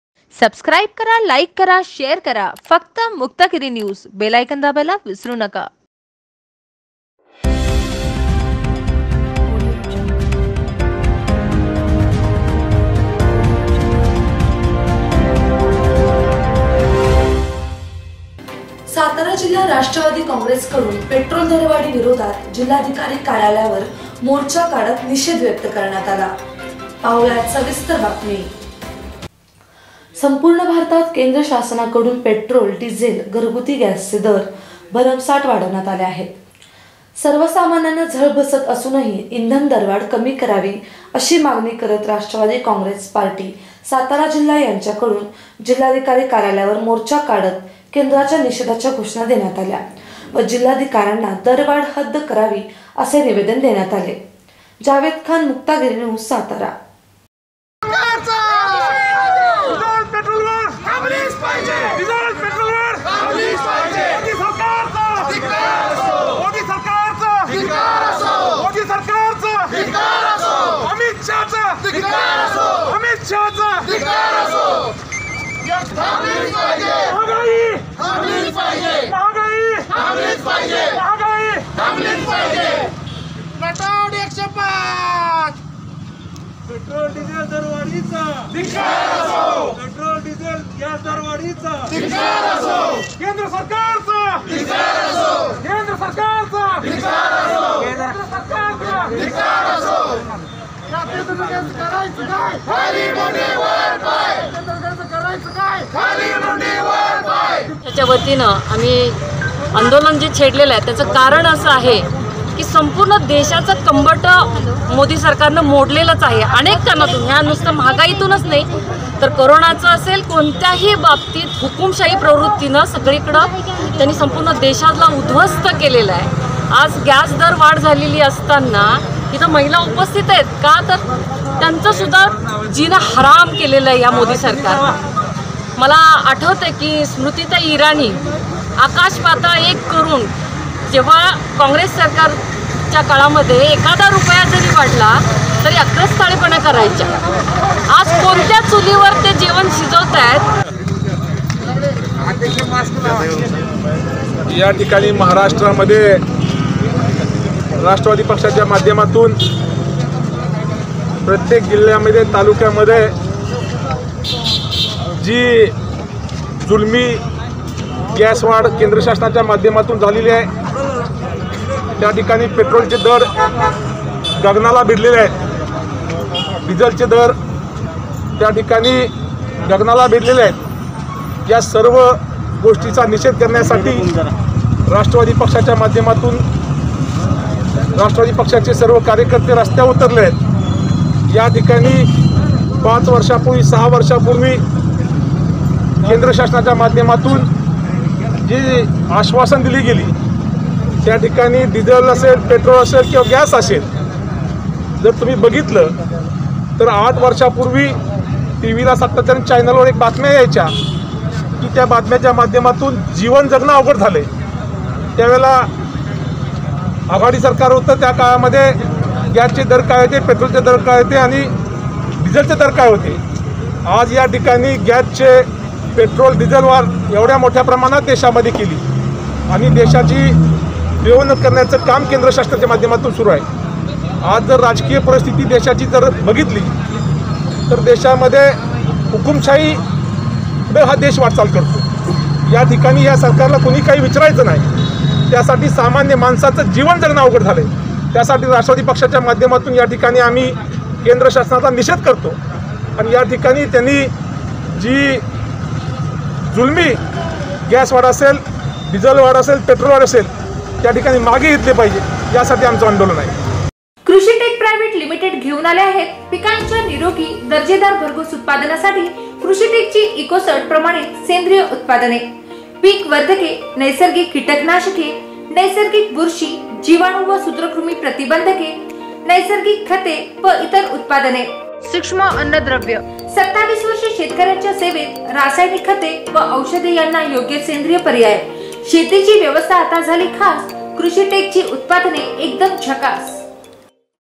इंधन दरवाढी विरोधात राष्ट्रवादीचे जिल्हाधिकारी कार्यालयासमोर आंदोलन ..
इंधन दरवाढी विरोधात राष्ट्रवादीचे जिल्हाधिकारी कार्यालयासमोर आंदोलन  ..  रोजच्या ताज्या बातम्या पाहण्यासाठी आजच लाईक करा शेअर करा आणि सबस्क्राईब करा फक्त मुक्तागिरी न्यूज ... बेल आयकॉनवर क्लिक करायला विसरु नका ...  प्रतिनिधी - जावेद खान सातारा ...